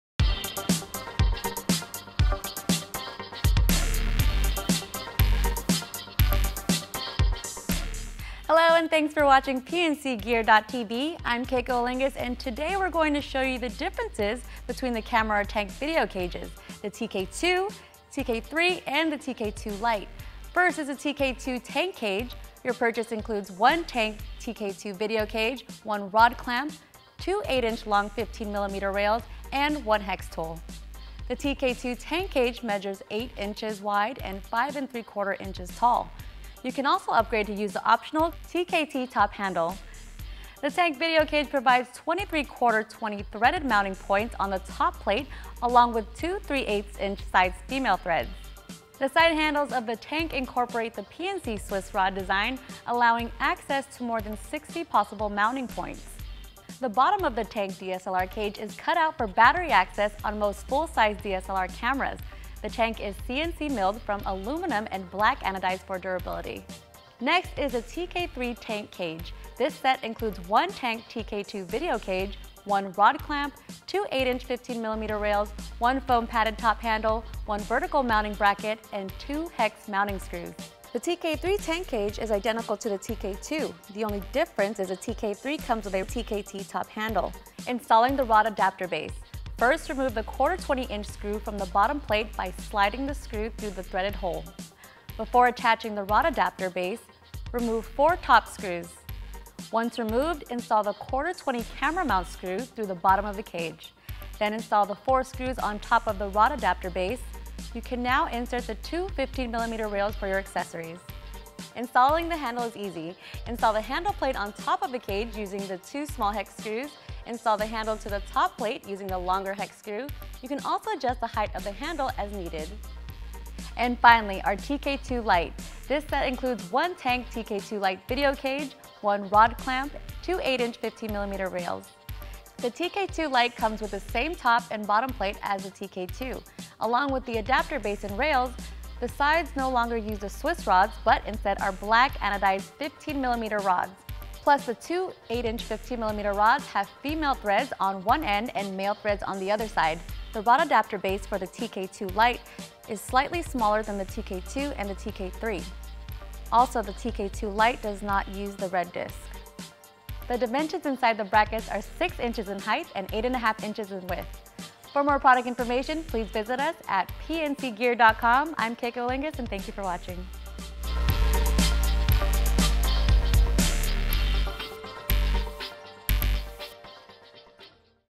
Tank Cage
The Tank DSLR video cage provides an infinite combination of mounting points for virtually any setup you can think of. A great platform for mounting preview monitors, microphones, sound recorders, lights, and more.

The bottom of the Tank DSLR cage is cut out for battery access on most full size DSLR cameras. The Tank is CNC milled from aluminum and black anodized for durability.